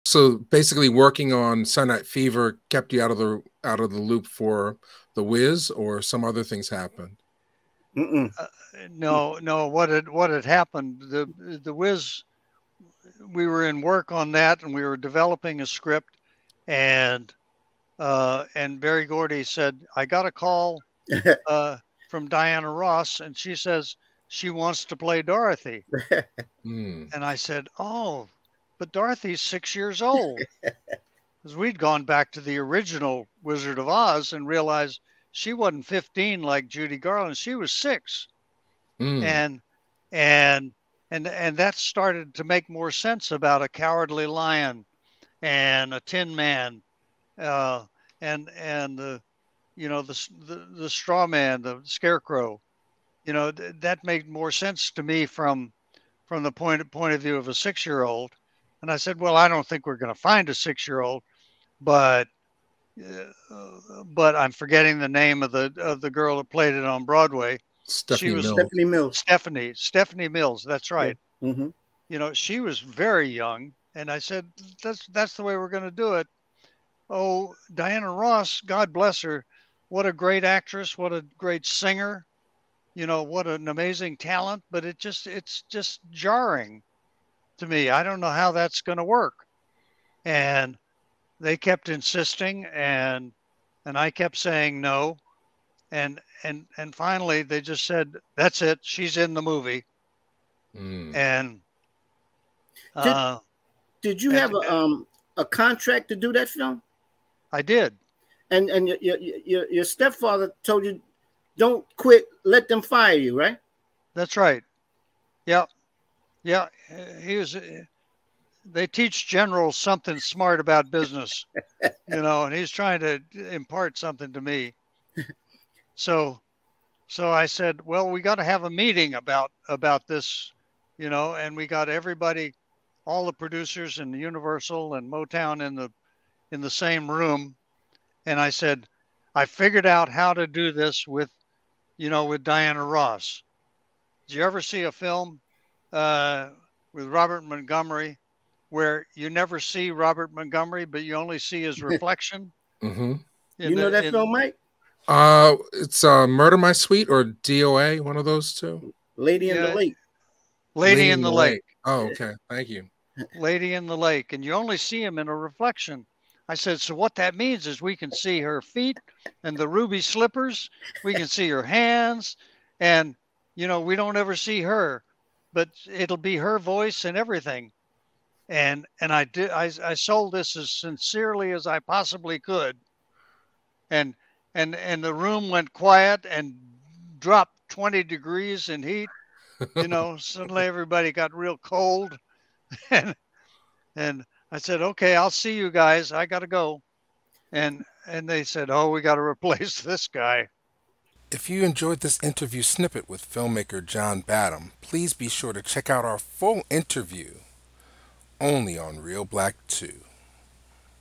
John Badham - "Why I Left The Wiz" (2023) | Interview Snippet
Mr Badham's book, On Directing is available now wherever books are sold.
Reelblack's mission is to  entertain, educate, enlighten, and empower through film.